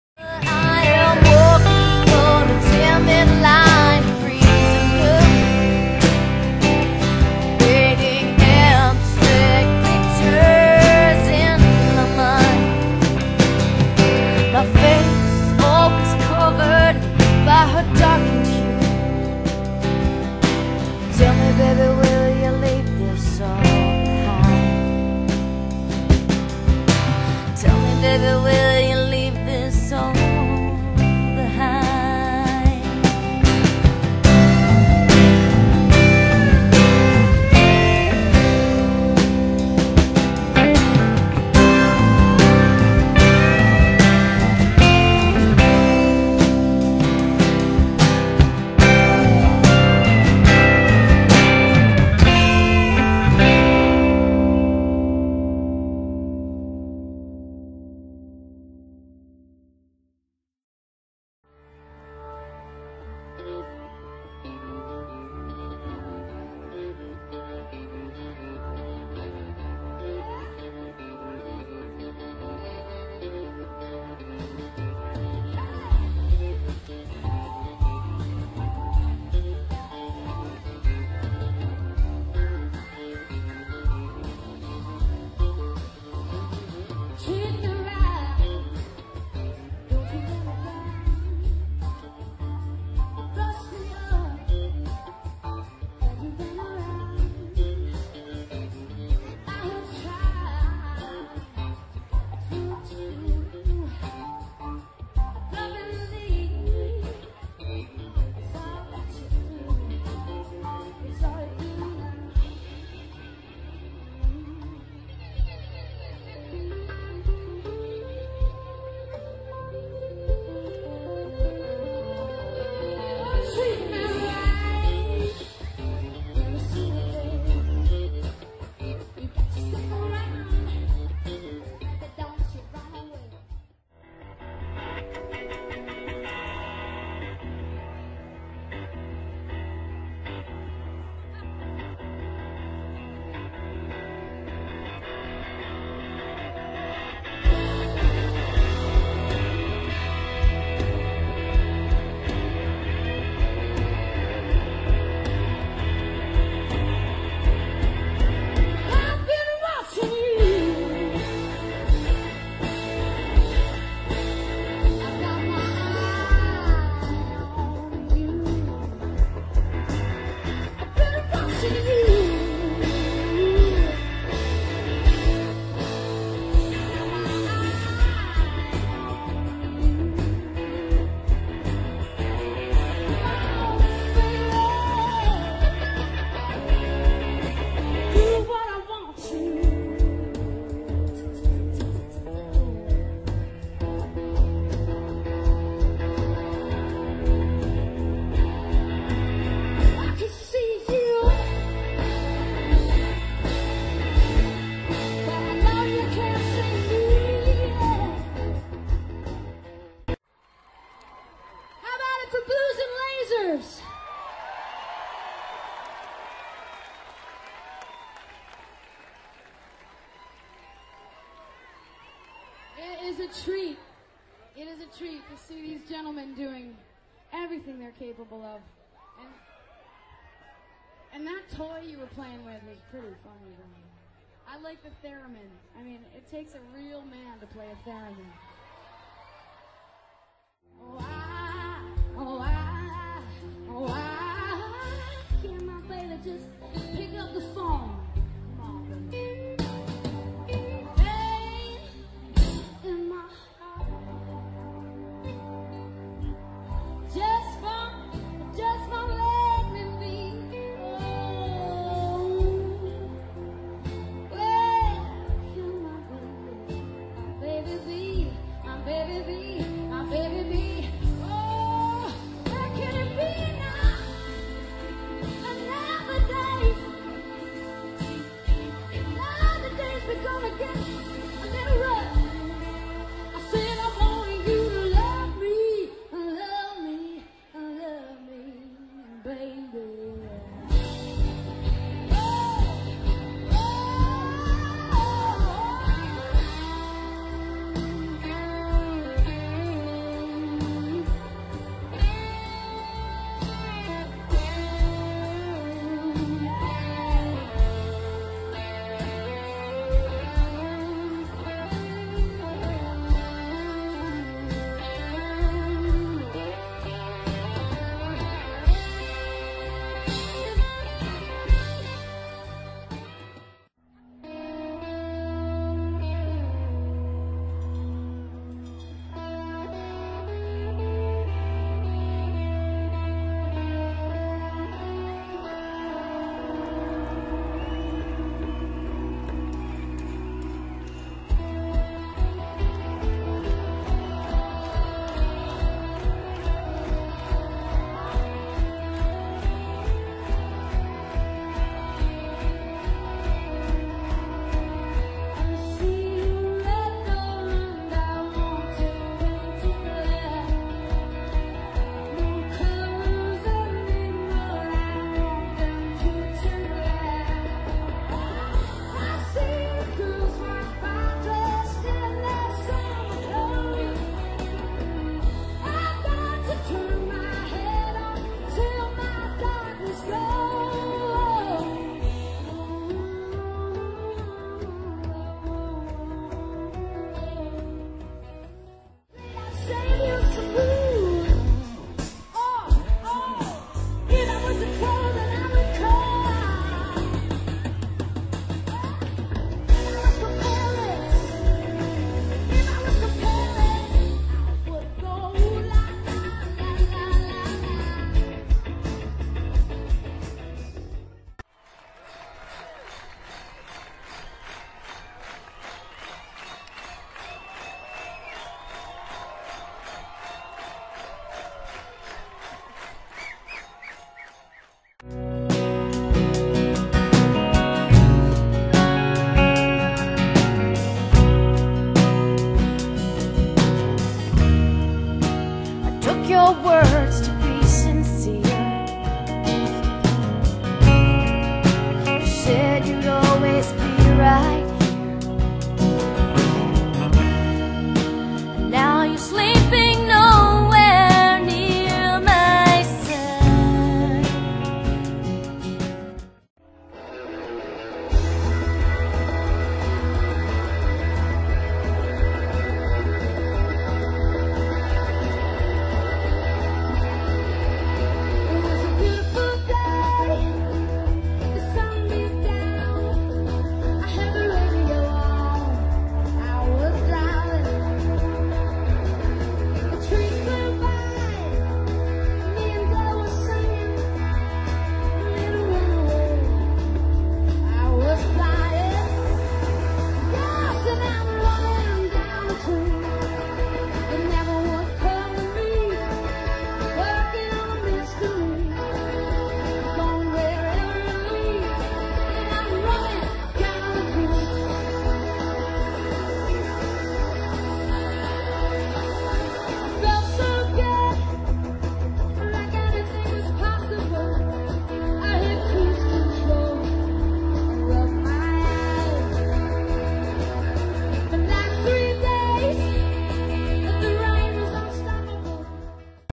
Lake Champlain Maritime Festival 2008
Grace Potter and the Nocturnals, Blues and Lasers and the Leaves take the stage and the Potterville Posse Rocks the Docks